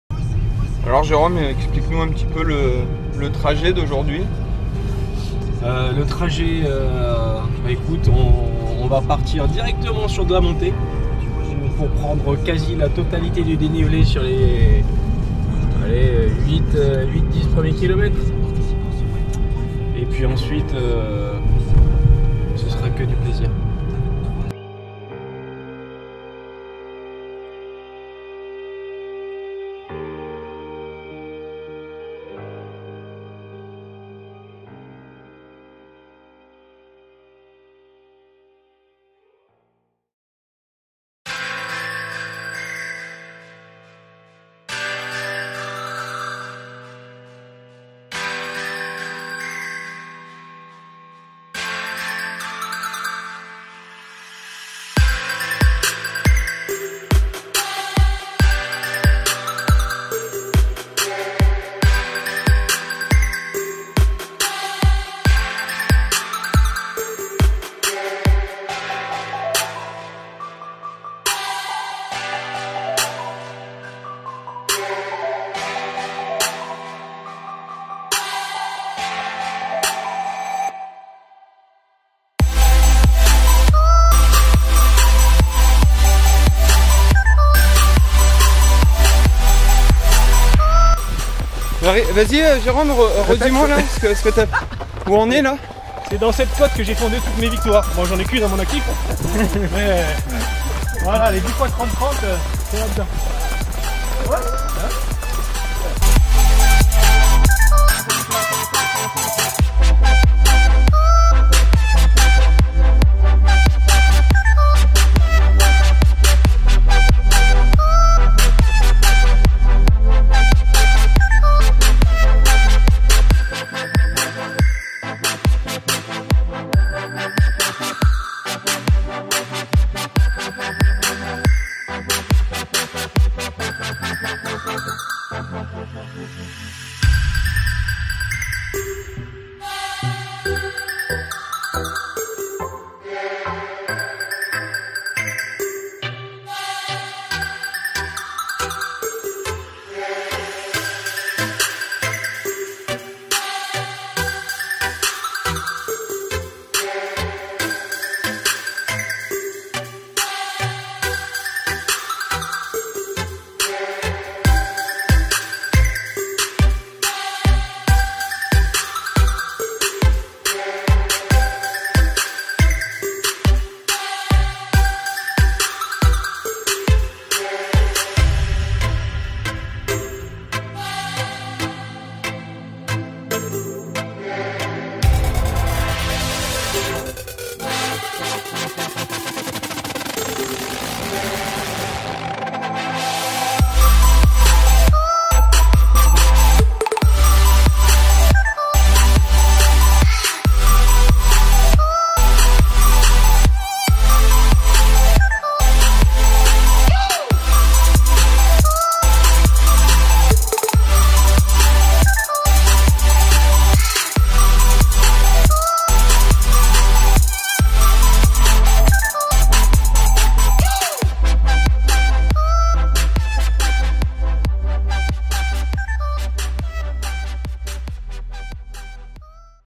TOWN TO TRAIL | Cannes "From the sea to the top" | Feat. François Satier
April 2016. Here is the latest Trail to Town . At Cannes this time with a great team . We did two different loops starting from the showroom lululemon athletica . We discovered the hills above Cannes .